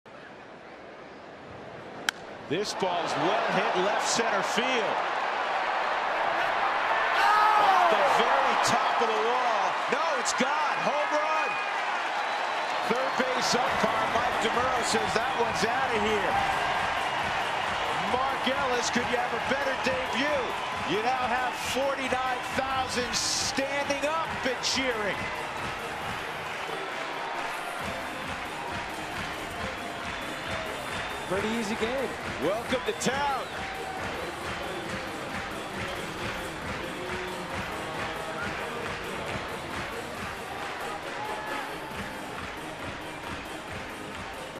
2011/07/01 Ellis' two-run homer
KC@COL: Ellis hits a two-run homer to left-center